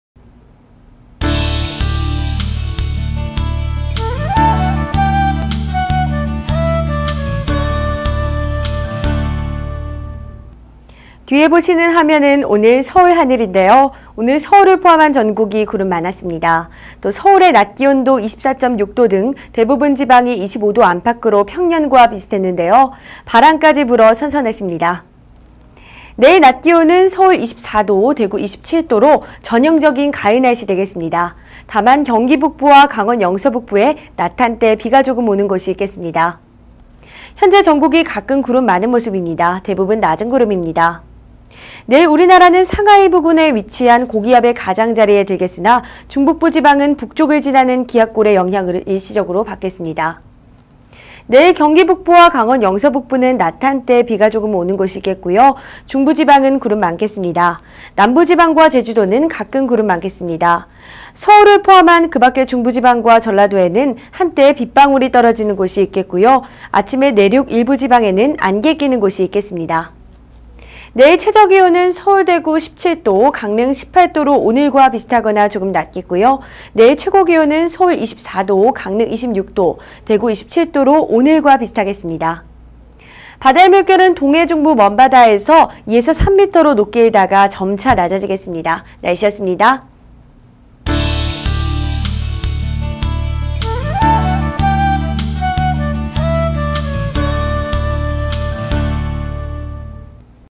[날씨정보 9월 18일 17시 발표]
-내일(수) 경기북부*강원영서북부 낮 한때 비 조금
           중부지방 구름 많음
           남부지방*제주도 가끔 구름 많음
           서울을 포함한 그 밖의 중부지방*전라도 새벽~ 낮 빗방울
           아침 내륙 일부지방 안개
           바다의 물결 동해중부먼바다 2~3m로 높게 일다가 점차 낮아지겠음